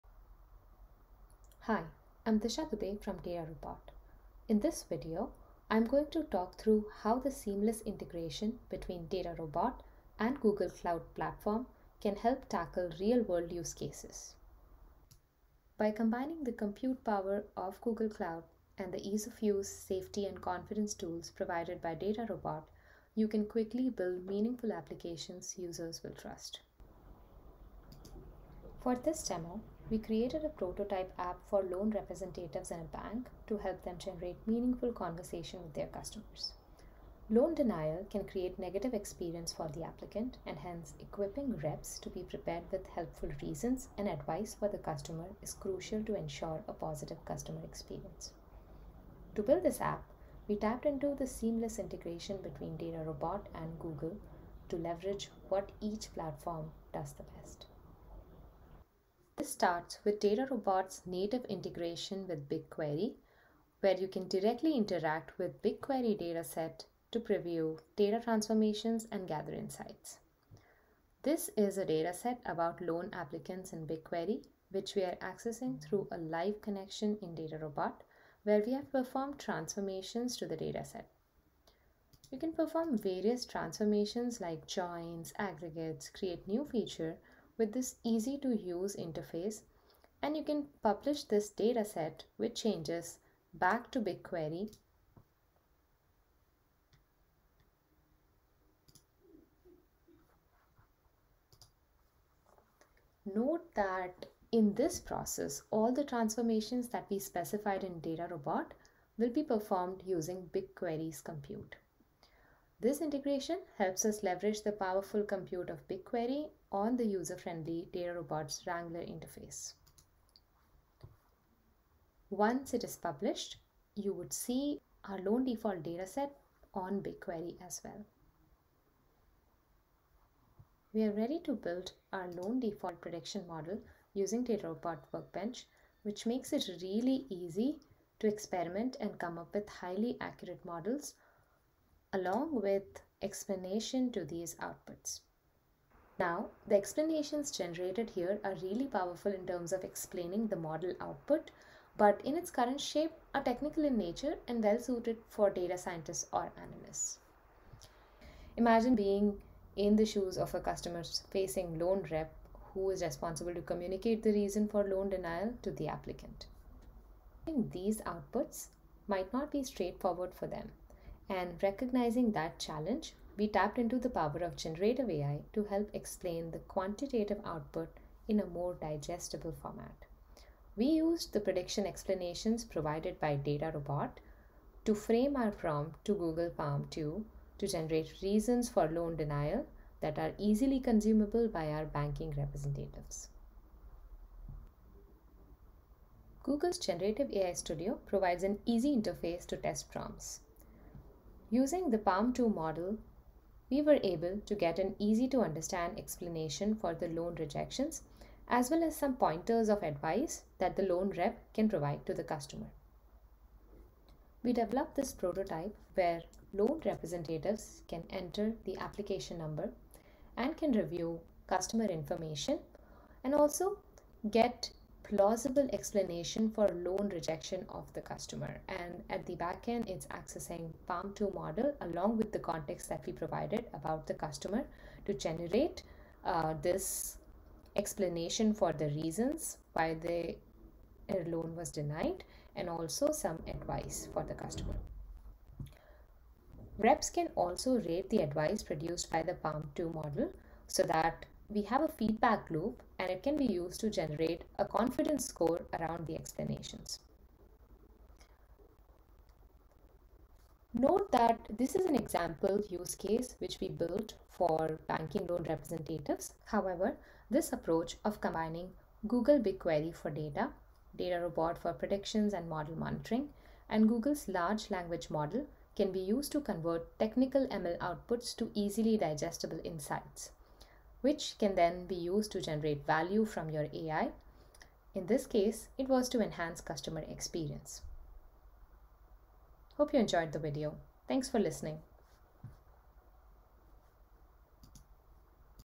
DataRobot + Google BigQuery and PaLM 2 + Streamlit | Loan Denial Explanations
This is a demonstration of a joint Generative AI solution combining DataRobot and Google Cloud Services unveiled at Google Cloud Next in August 2023. The end-to-end application is built in a matters or hours or days leveraging best-of-breed components from DataRobot and GCP.